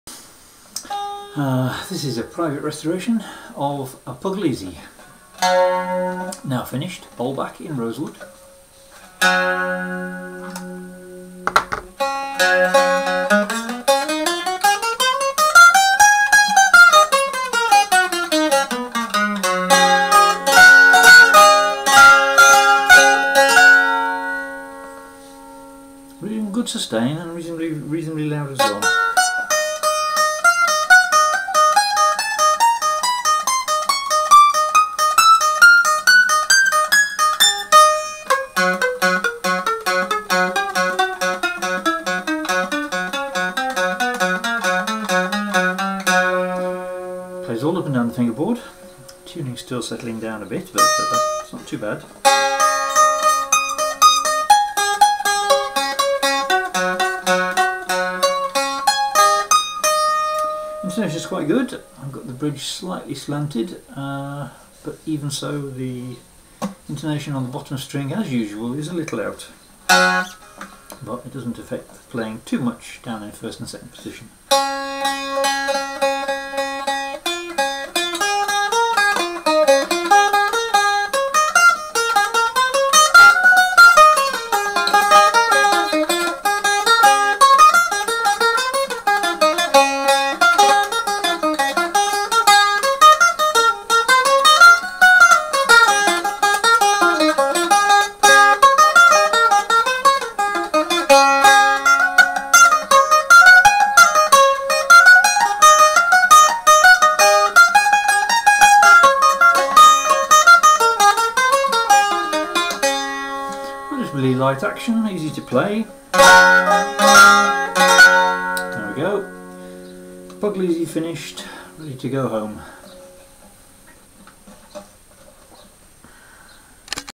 Puglisi bowlback prvt
This is a private restoration or an old Puglisi bowlback, dated 1912. Restrung with GHS ultralight strings, on a 332mm scale, it is playing well with fair volume and good sustain.. :)